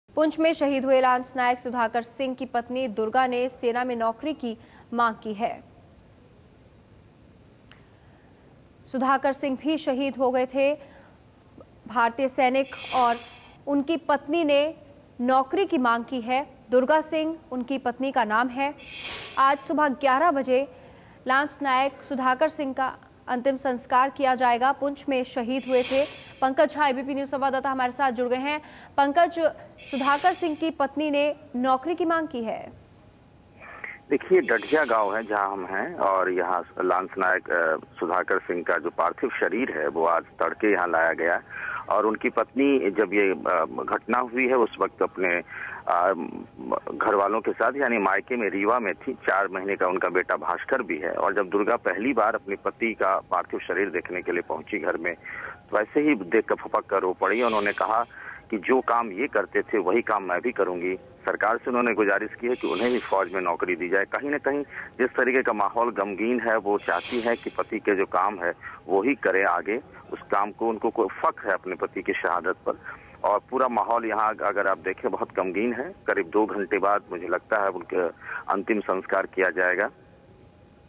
Wife of martyr Sudhakar Singh demands job after death of her husband
Durga Singh, wife of Lance Naik Sudhakar demanded job in Army after the death of hr husband. Sudhakar was assassinated by Pakistan army in Punch sector at LOC in Kashmir.